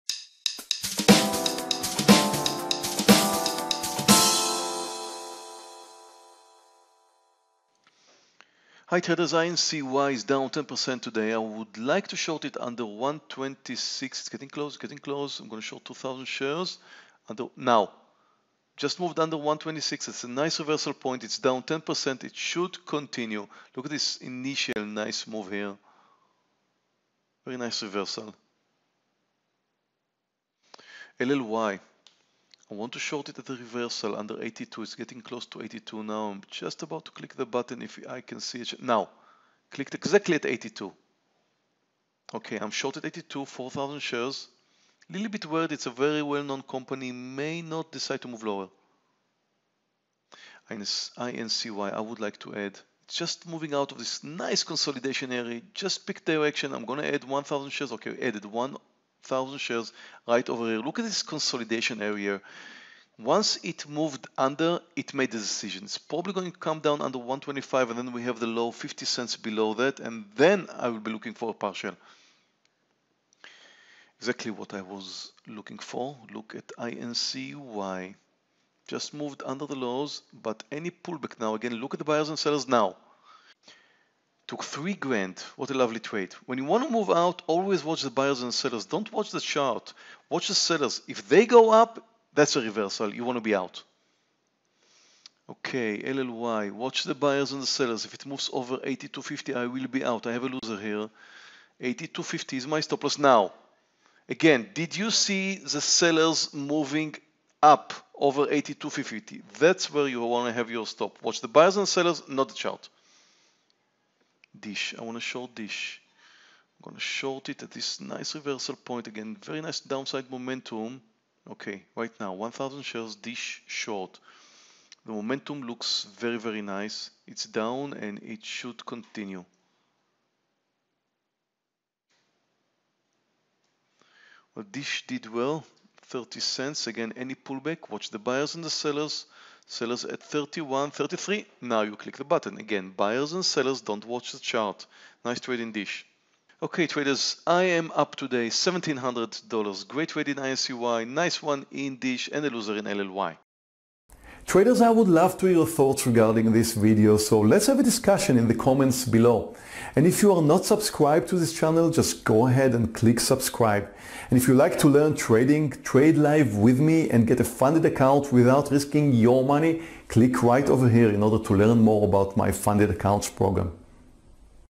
Live Day Trading - $1,700 in Profits - Meir Barak
Live Day Trading Stocks - How to make money by day trading stocks -  $1,700 in profits.
Meir Barak day trading live on April 17th. 2017.